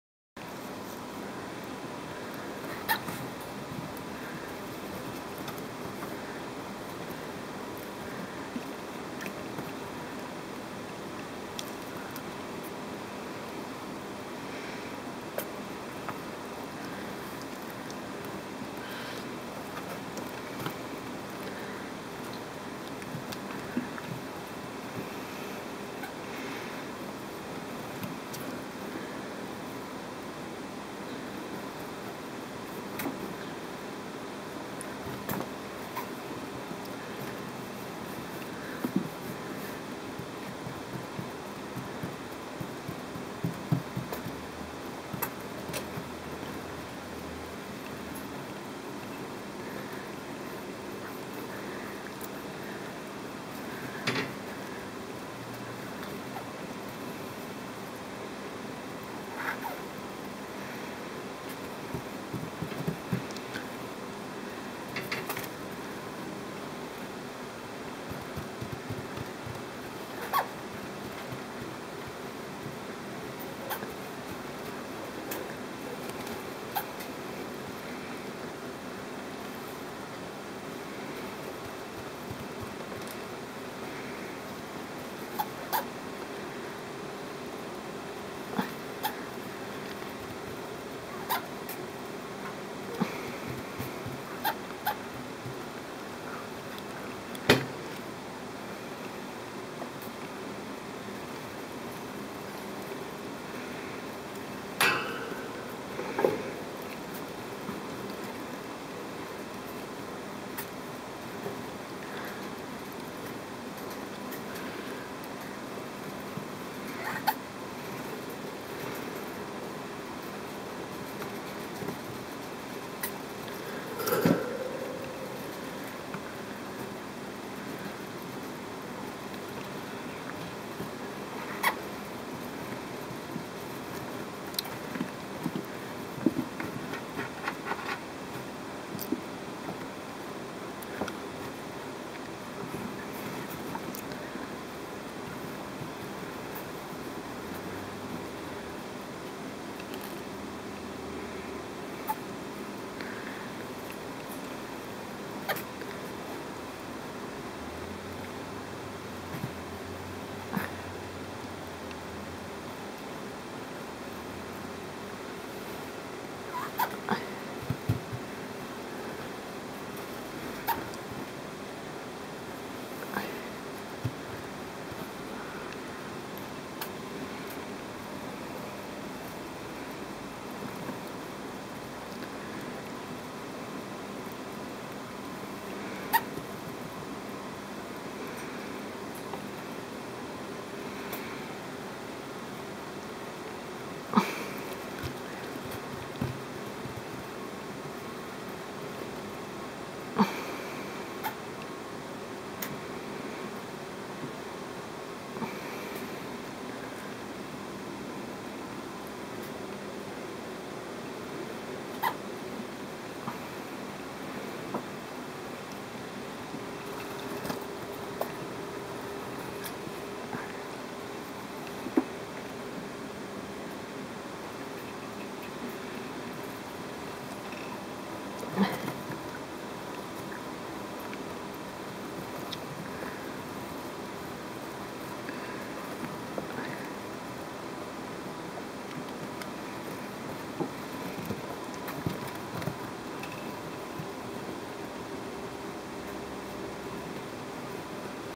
Rat Pup Hop
Playing with my 6 week old dumbo rex rats and their year-old second cousin.